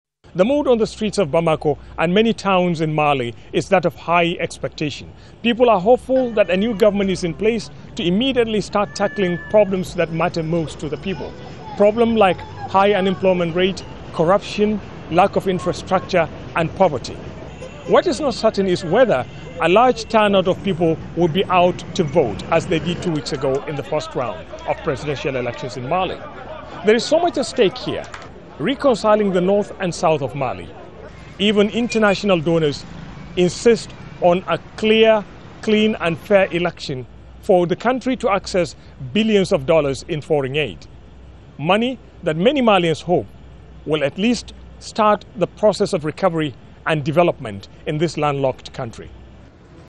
Malians gear up for presidential runoff
Voters in Mali are heading to the polls in the second round of the Presidential election.

It is an important step towards reestablishing democracy in the country after last year's military coup.

Al Jazeera's Ahmed Idris reports from the capital Bamako.